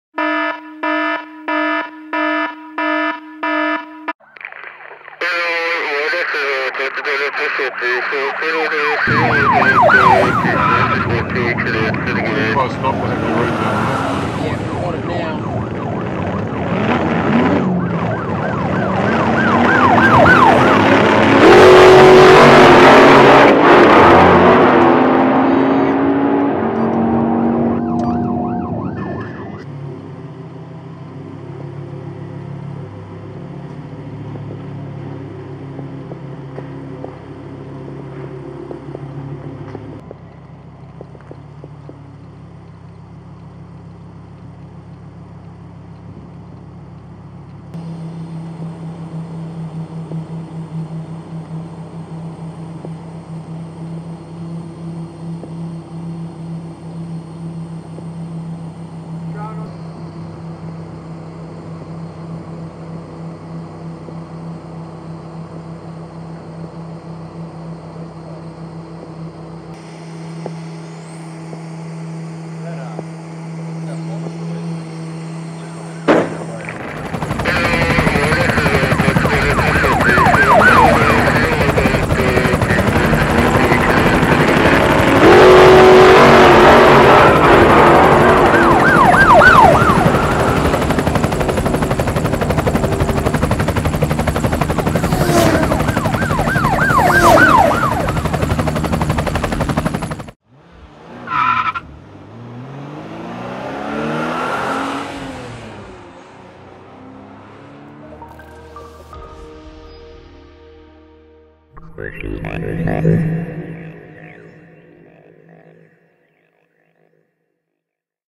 Police Chase Bitcoin Millionaire Lamborghini, Police Auction Lamborghini
Watch Police chase Bitcoin Rich Young Man Lamborghini, Watch the chase at the Start and end of the Video. A customised Lamborghini coupe seized by police under Queensland’s tough anti-hoon laws will go up for auction next week. Full Story
Oh we both receive $10 worth of Bitcoin*